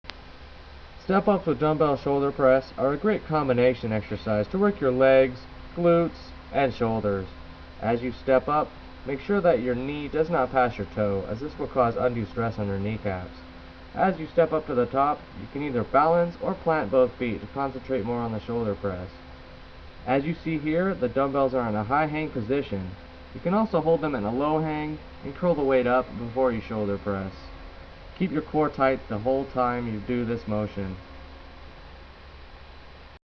Dumbbell Step Ups w/ Shoulder Press Combination Exercise
Personal Trainer Michael Behnken, MS, NASM-CPT-PES, CSCS

Combination exercises such as this one have a part in all fitness programs as they are truly functional movements.

The step-up can be a great add-on to many exercises.  This will not only exercise the big leg muscles which have a large role in increasing metabolism but will work on balance.